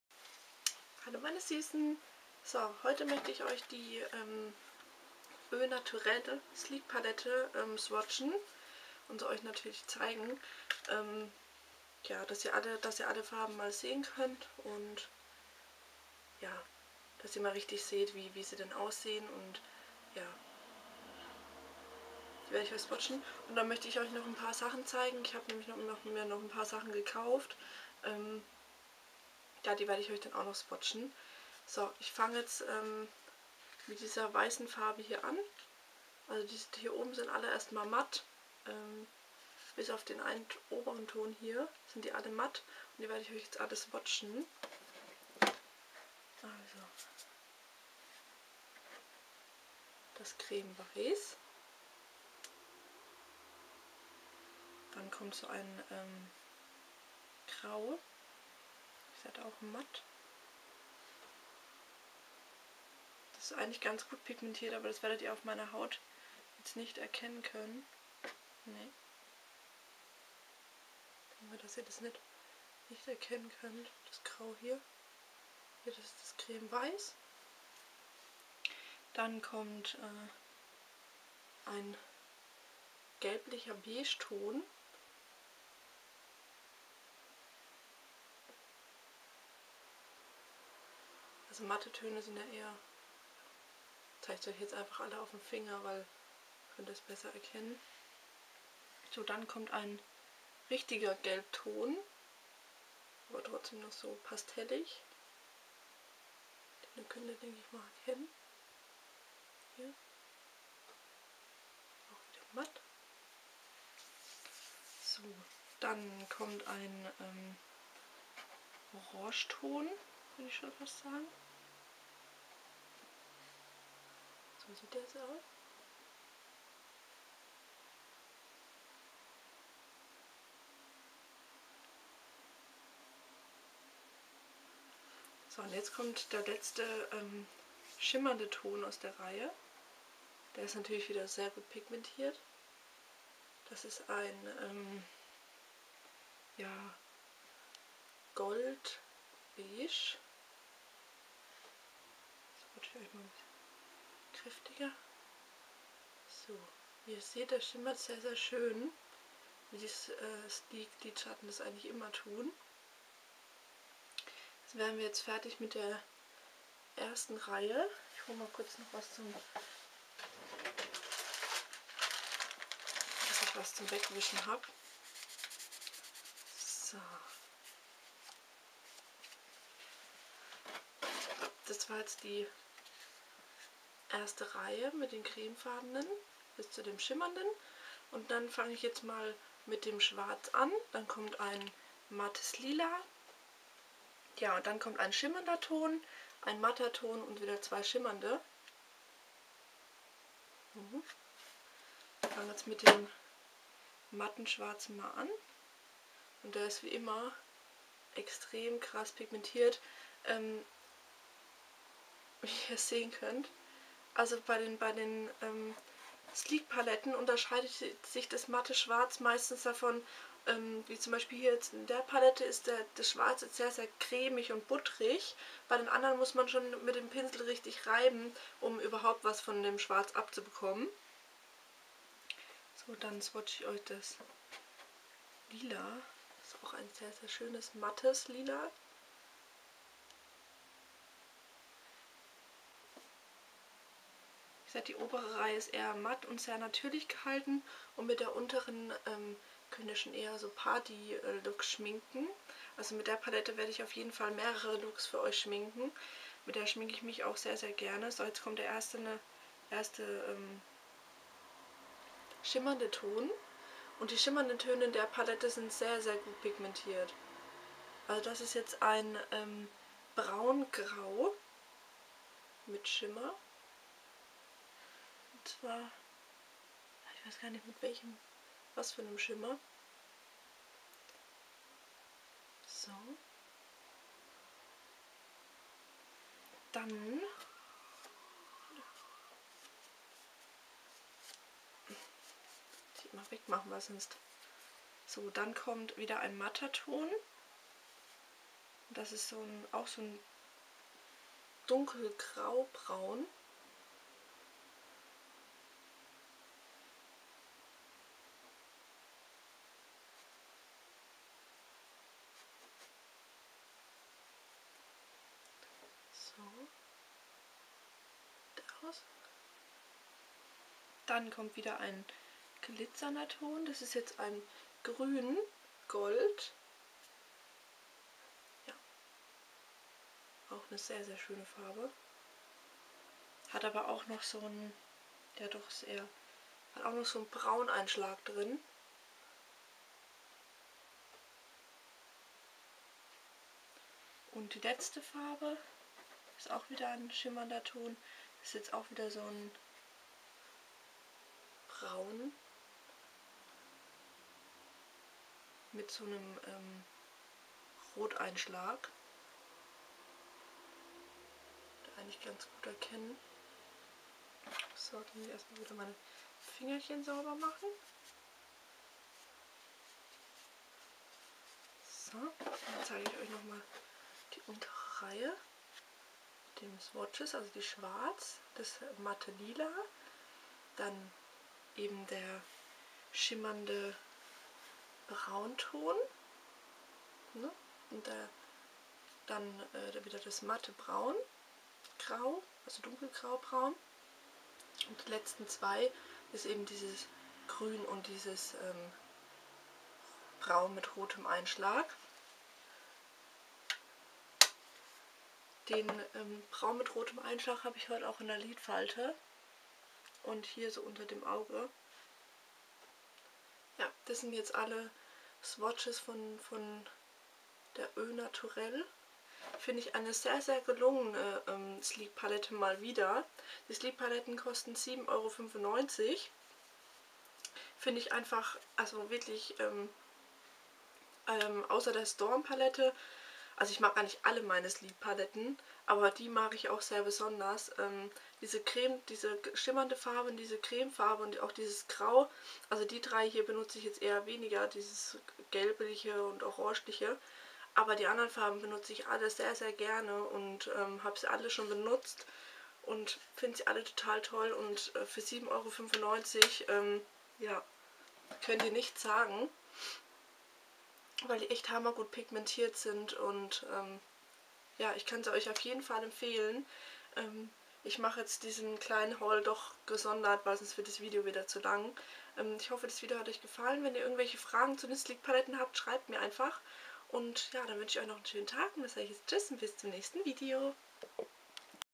Sleek Au Naturel + swatches
Hier seht ihr die Sleek Au Naturel Palette mit swatches ich zeige euch jeden einzelnen Lidschatten :) Viel Spaß :)